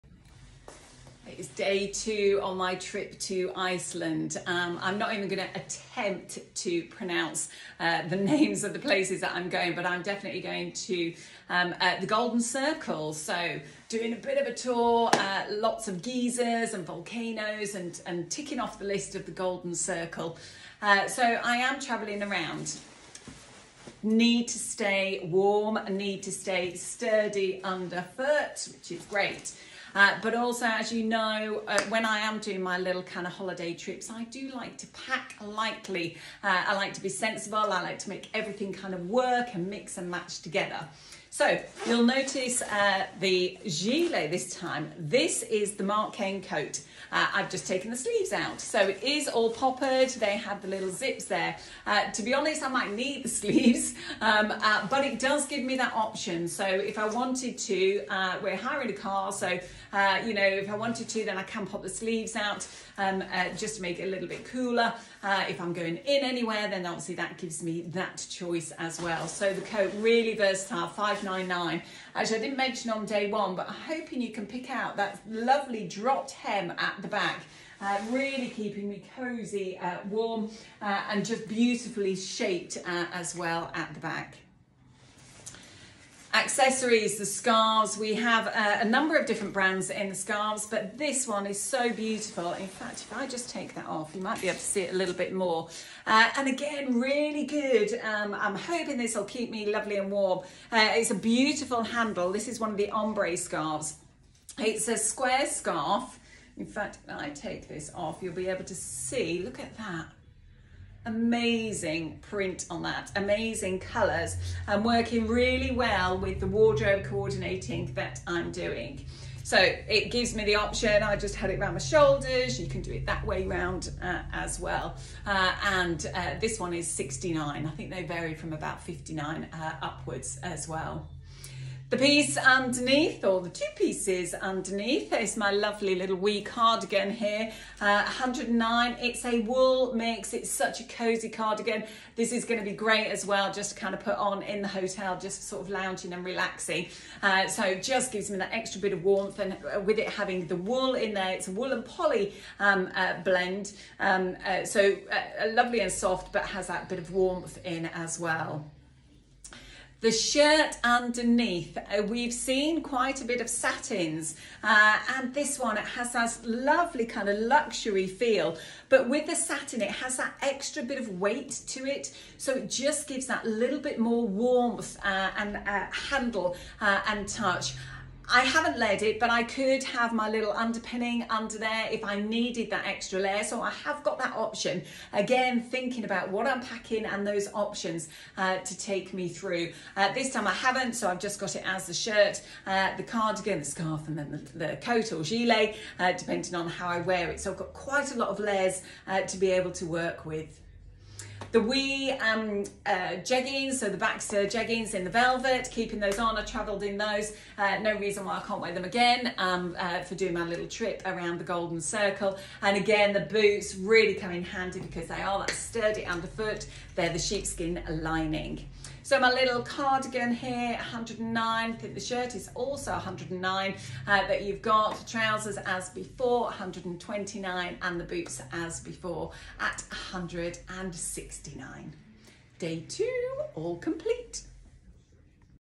Packing For A Mini Break to Iceland - Day Two
Heading out to explore the amazing scenery in Iceland. Determined to stay warm from top to toe. Using my carefully packed case to get maximum wear out of my clothes.